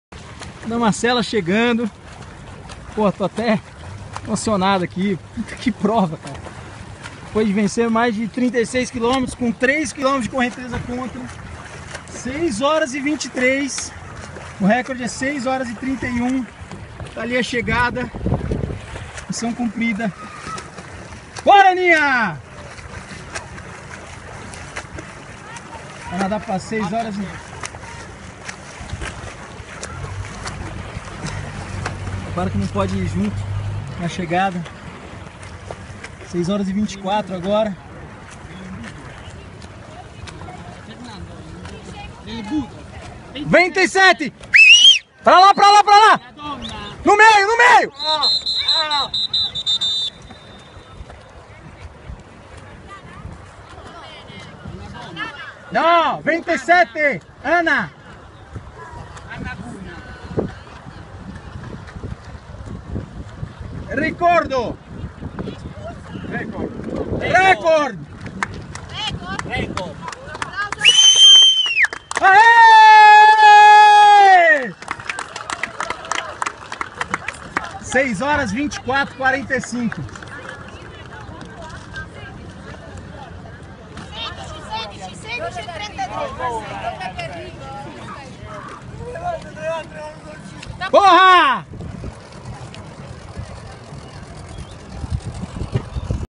Ana Marcela Cunha é recordista da travessia Capri-Napoli
Assista à chegada da atleta